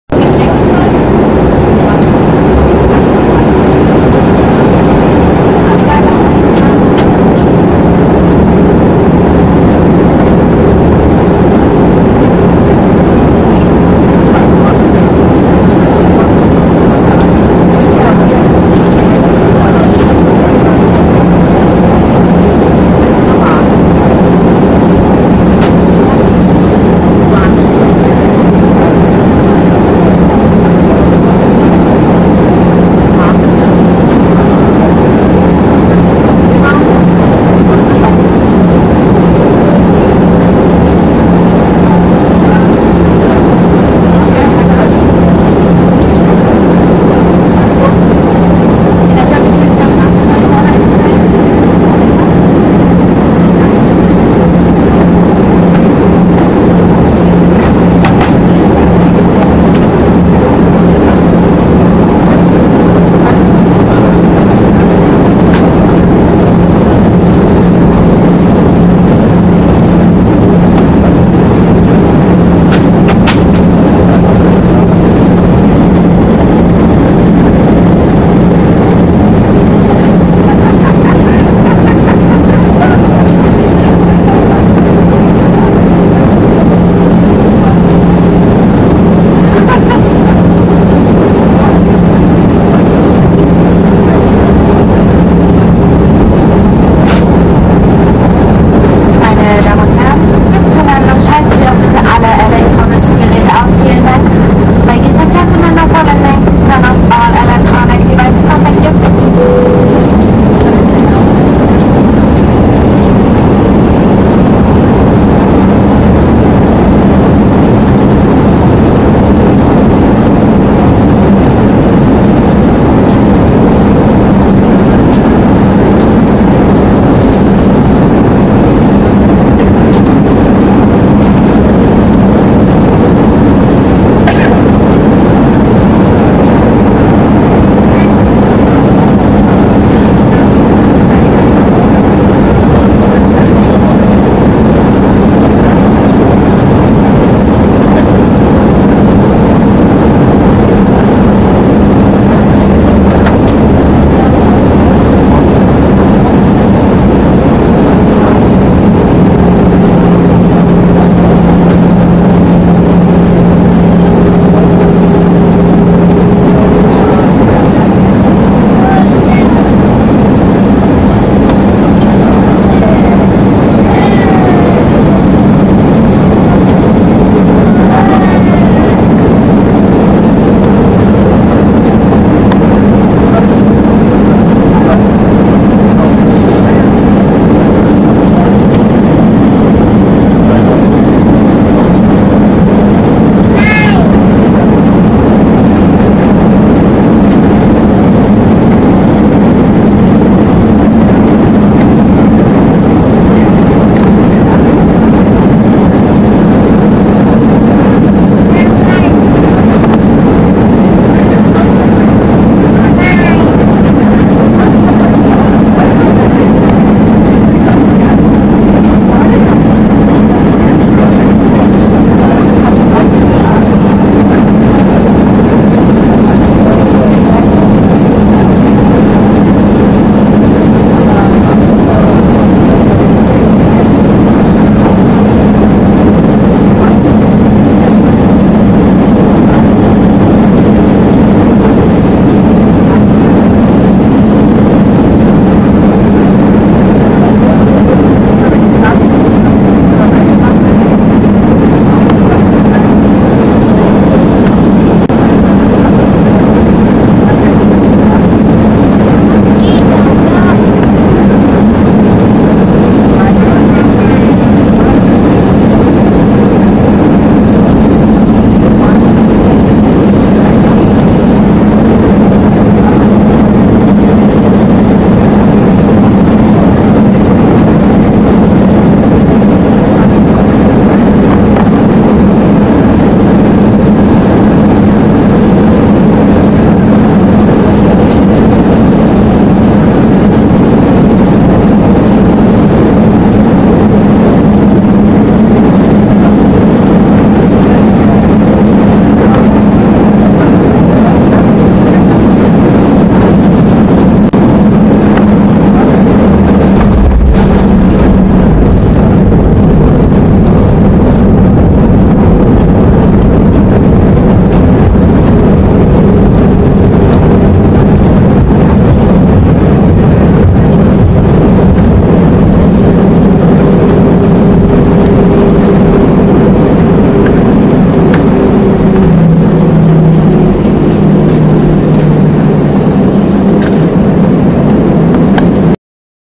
Arrival Berlin Tegel Airport
This is what you see when you arrive at Berlin Tegel Aiport (TXL) and you have a nice flight around Berlin.